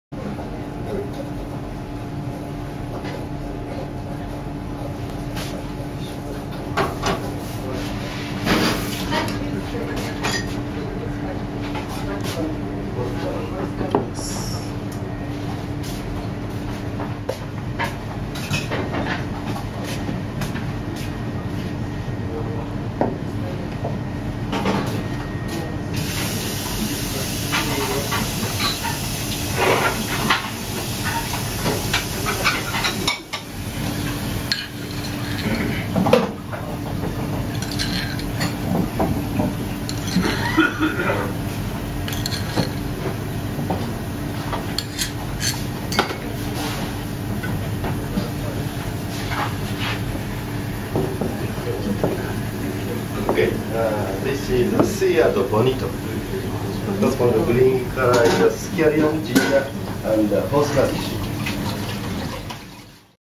Sushi Yoshitake - Bonito
Bonito is a fish not often served raw as it spoils quickly, but this dish was impeccable - barely seared tataki style, doused with a bit of soy sauce, and topped with a mound of finely minced green onion and ginger.

Sushi Yoshitake, which current holds 3 Michelin stars, originally opened in 2004, its owner and head chef being Masahiro Yoshitake, who trained for 28 years in various restaurants before opening his own place in Roppongi, moving to Ginza in March 2000. His rice is prepared with red vinegar in the “Edomae” way, a style dating back to the Edo period more than 150 years ago. He directly buys 70% of his fish rather than using the main market. His sea urchins are sourced directly from Karatsu, octopuses and sea breams are from Akashi, Hyogoken and Hagi, Yamaguchiken. The sushi bar itself is located on the third floor of a nondescript building in a side street in the Ginza area.